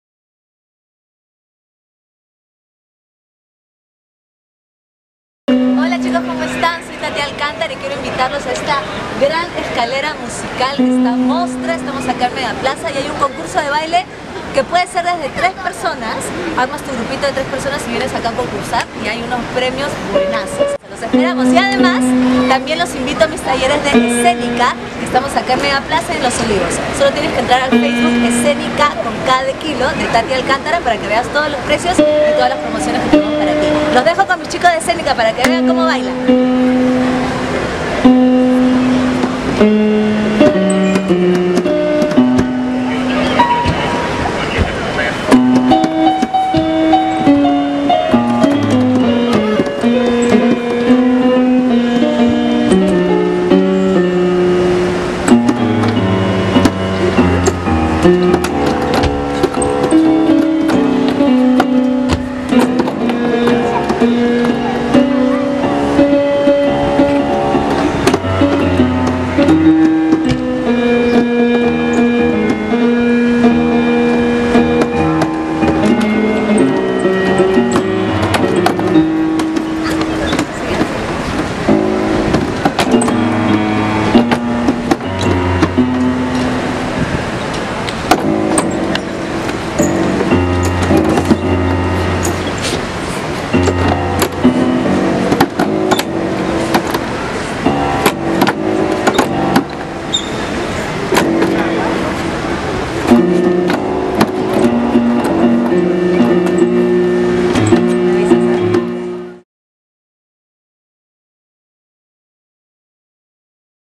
¡Tati Alcántara te invita a bailar en la Escalera Musical!
¿Te apasiona el baile? Registra a tu equipo de tres personas para participar: Y podrán GANAR 02 guitarras acústicas y 01 teclado.